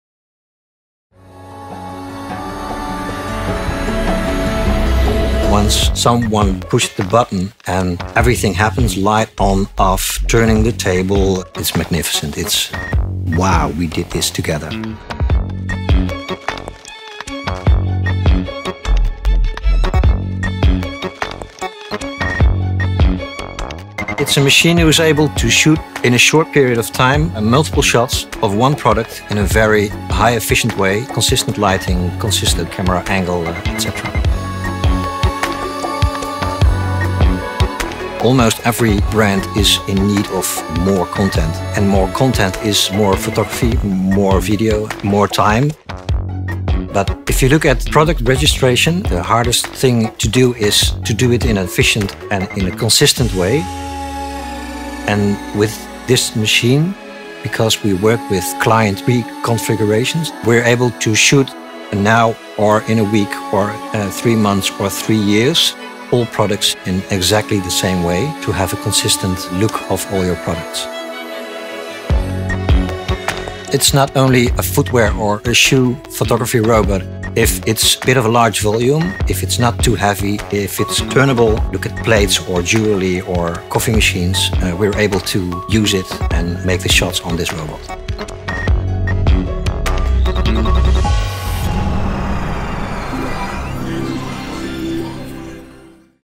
Lukkien Product Photography Robot
Innovation is embedded in Lukkien's DNA. Therefore, we have developed a product photography robot for the e-commerce market with which we answer to the growing demand for volume, quality and consistency regarding pack shots.